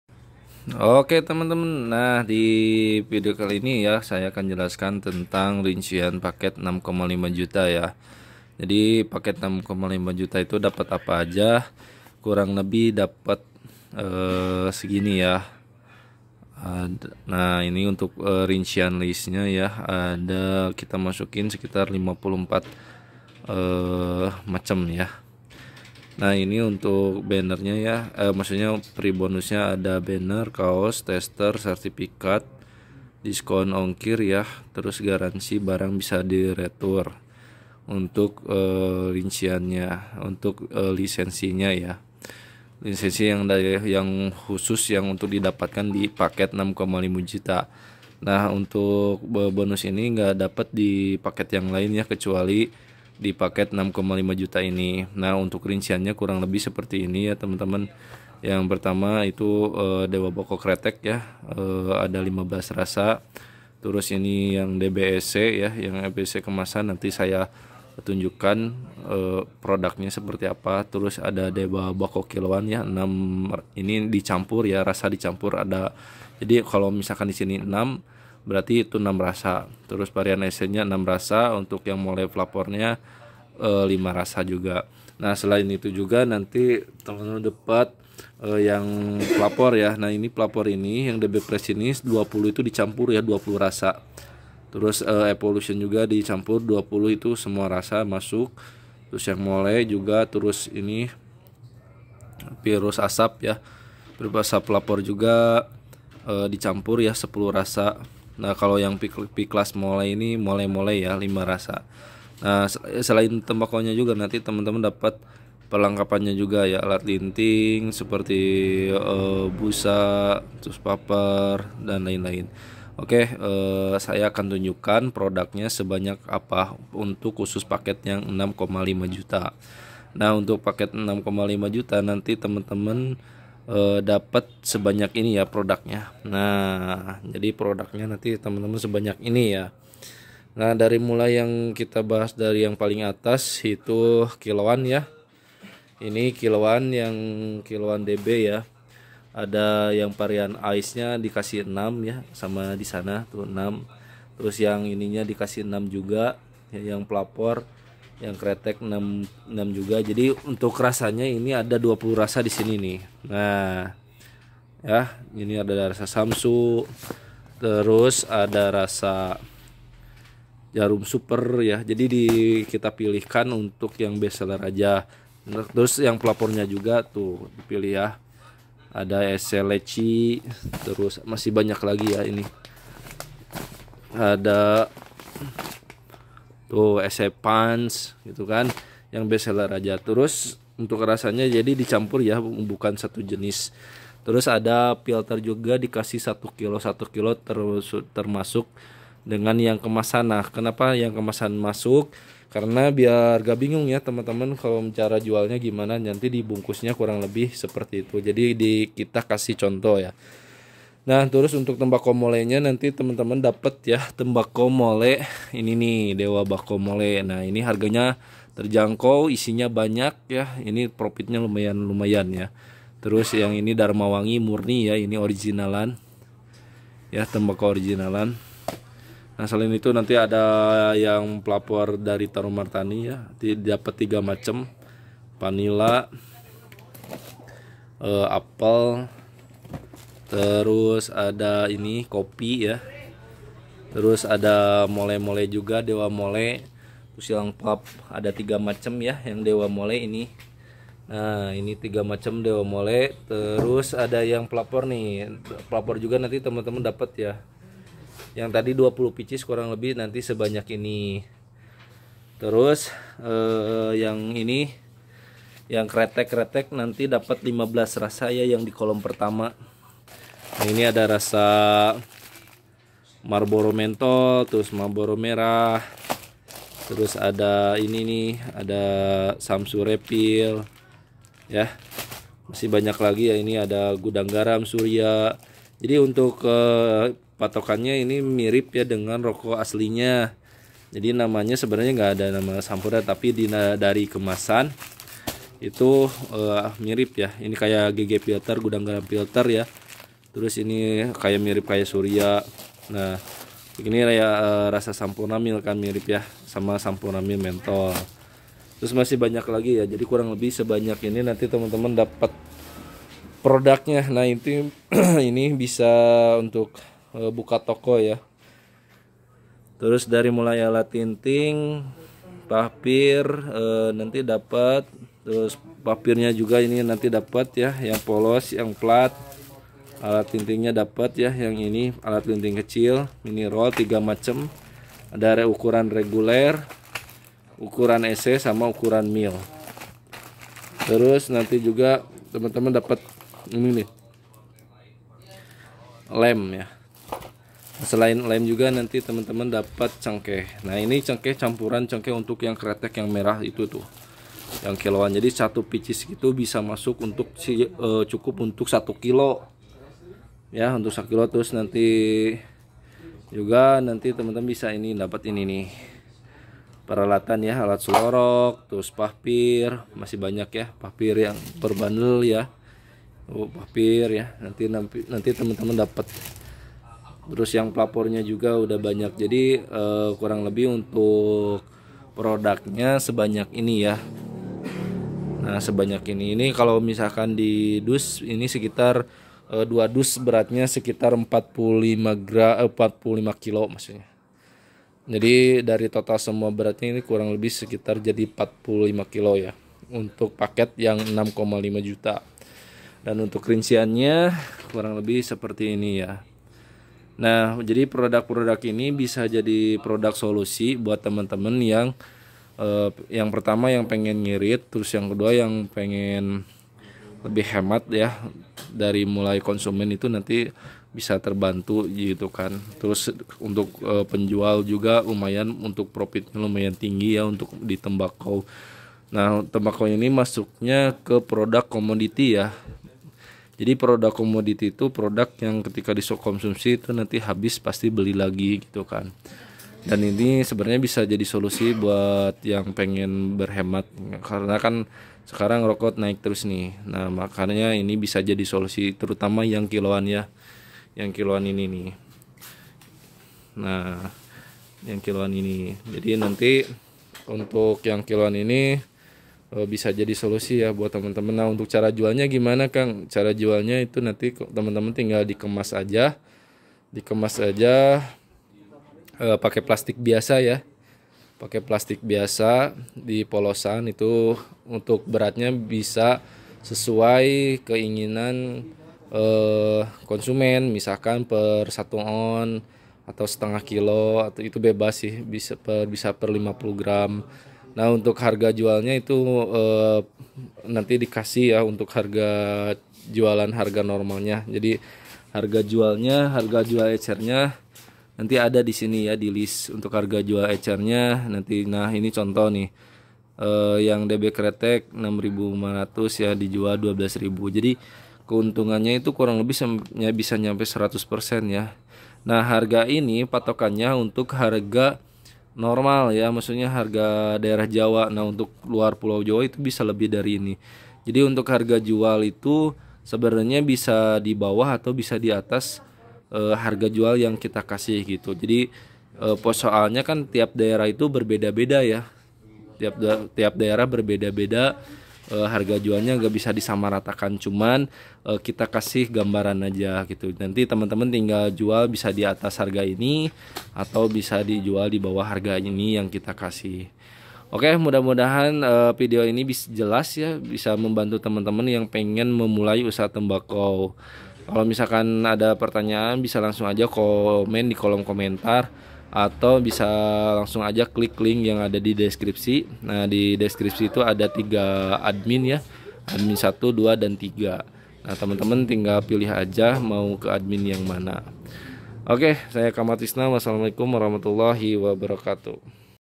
Peluang Usaha Tingwe Program Buka Toko Modal 6,5 Juta , Usaha Yang Lagi Viral !!
Assalamualikum wr wb apakabar semua? Mudah2.n dalam kondisi yang baik..
Seperti biasa saya akan share info tentang peluang usaha.

INFO PRODUK BISA HUBUNGI ADMINT DB
LANGSUNG AJA KLIK LINK INI